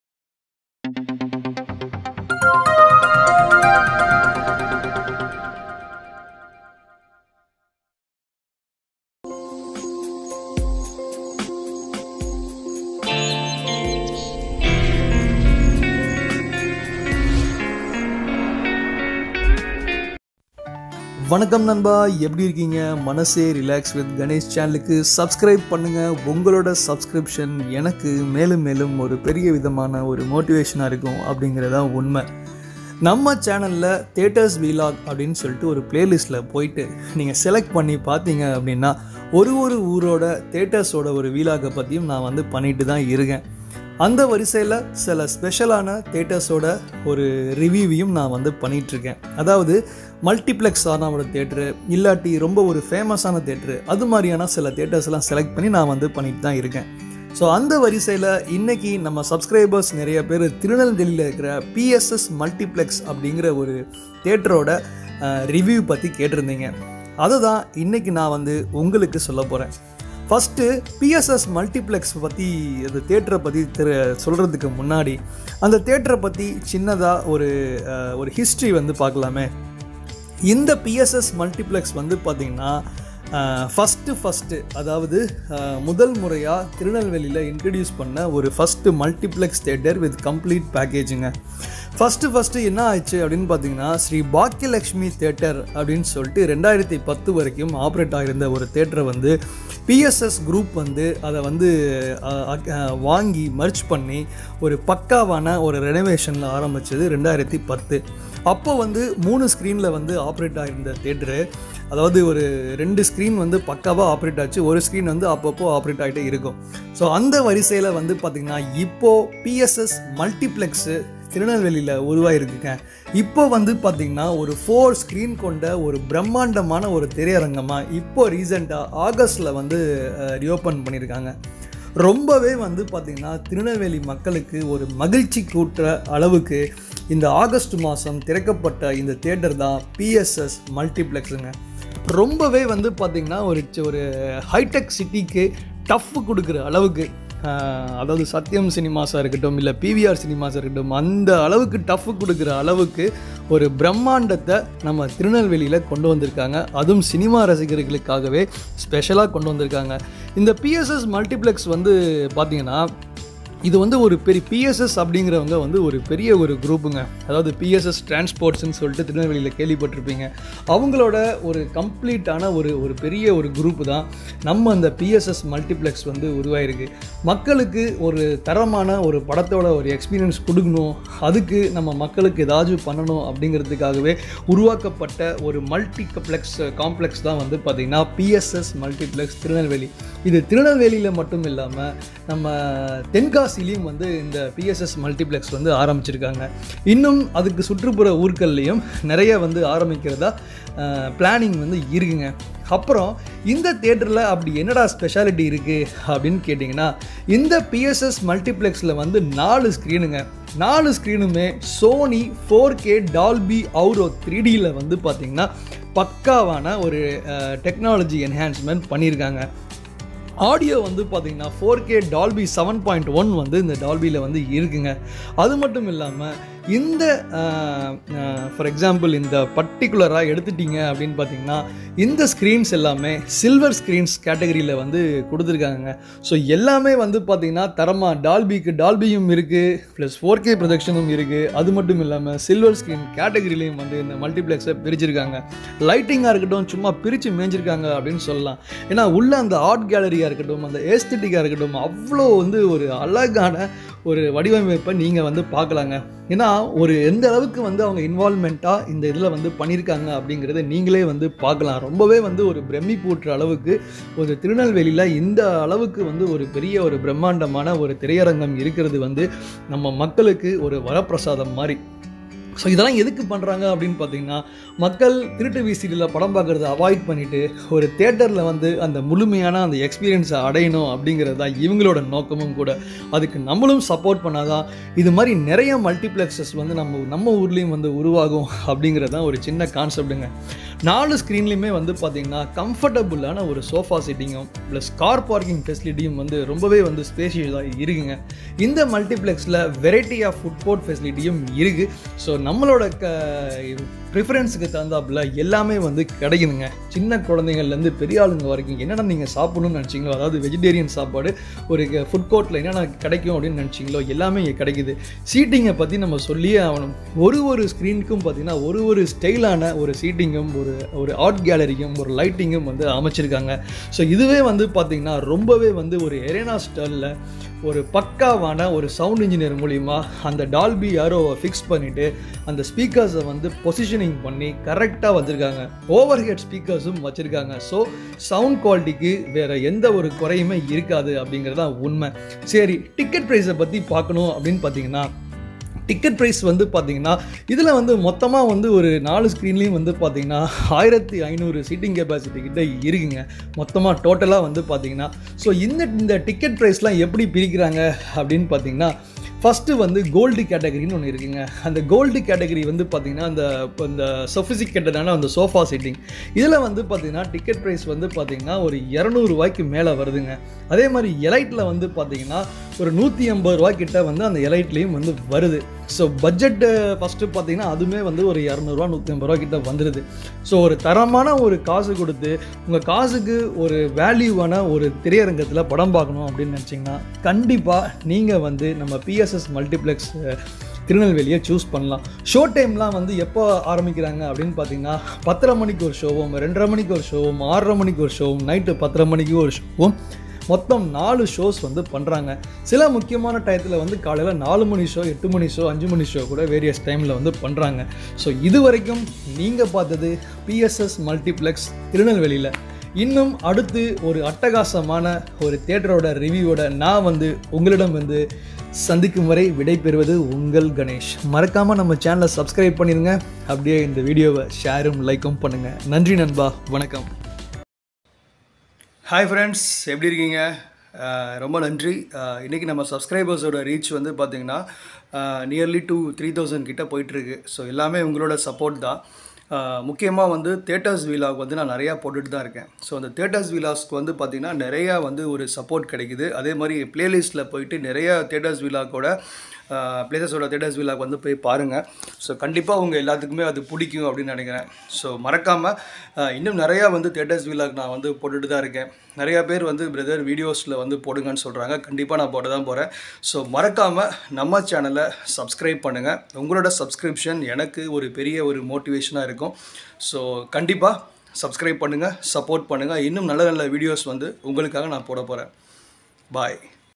PSS Multiplex Tirunelveli History and Full Tour | Best Multiplex in Tirunelveli
PSS Multiplex Dolby Atmos - Vannarpettai is a popular theatre located at South Byepass Road, Near DNA Labs India, Vannarpettai, West, Tirunelveli. PSS Multiplex Dolby Atmos - Vannarpettai has 1 screens. Movies now showing at PSS Multiplex Dolby Atmos - Vannarpettai are Naai Sekar Returns, Varalaru Mukkiyam, Gatta Kusthi and Baba. Facilities available at PSS Multiplex Dolby Atmos - Vannarpettai are Parking and Food And Beverage.